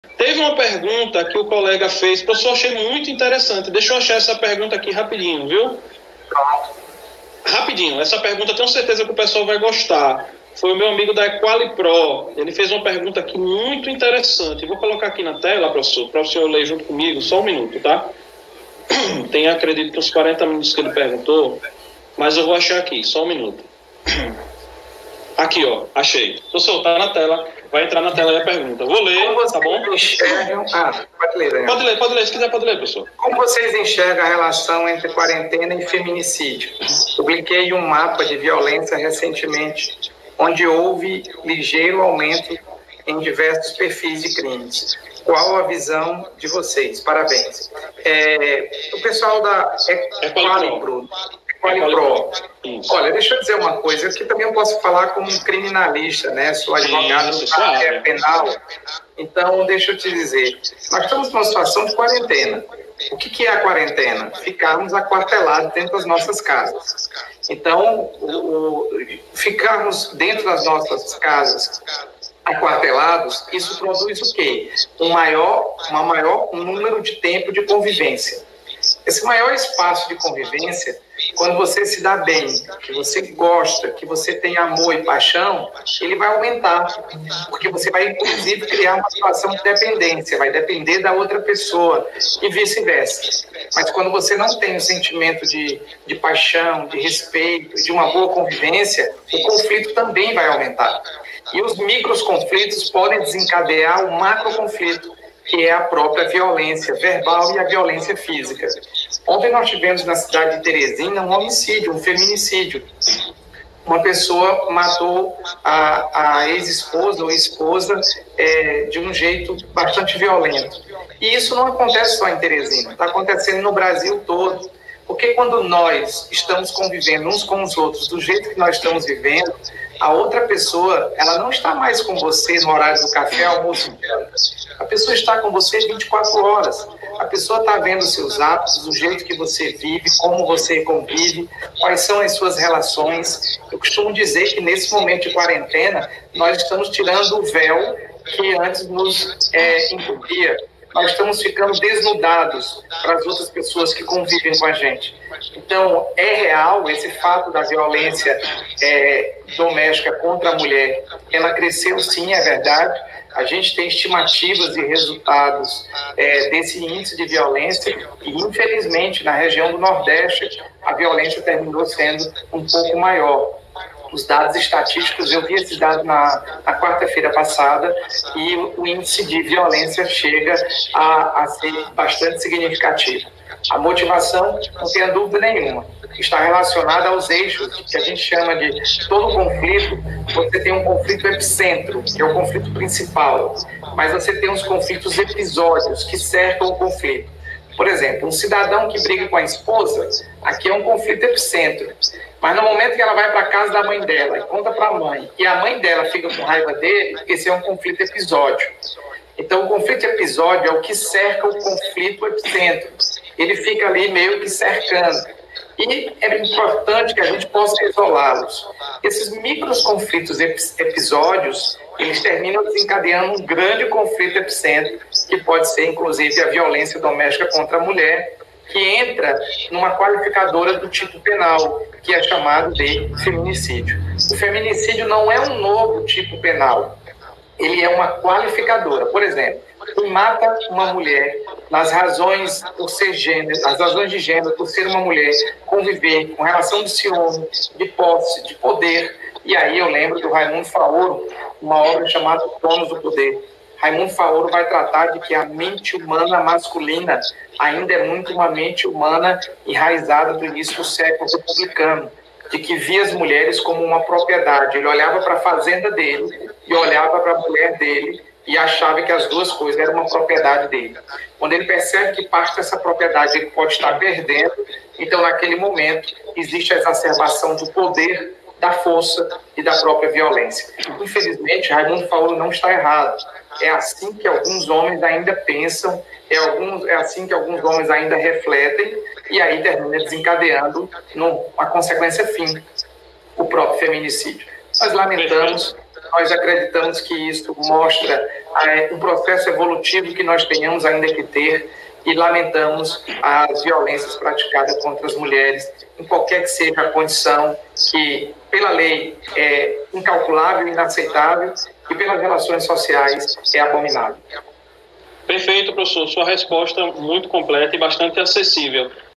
O Feminicídio em tempos de quarentena
Professor Alessander Mendes no Papo Condominial